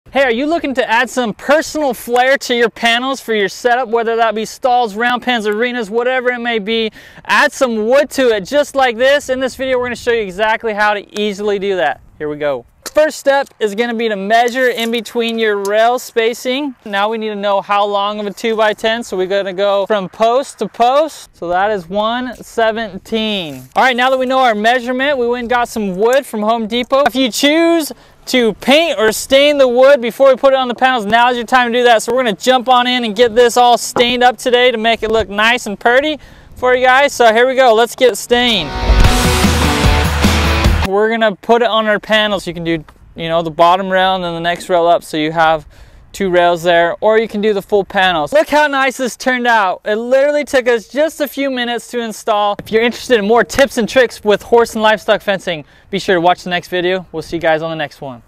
How To Easily Add Wood To Your Horse Panels!
Are you looking to upgrade your horse and livestock panels? Consider adding wood to your setup! In this video, we'll show you Step By Step on how to add wood to your horse and livestock panels with our NEW WOOD PANEL CLAMPS!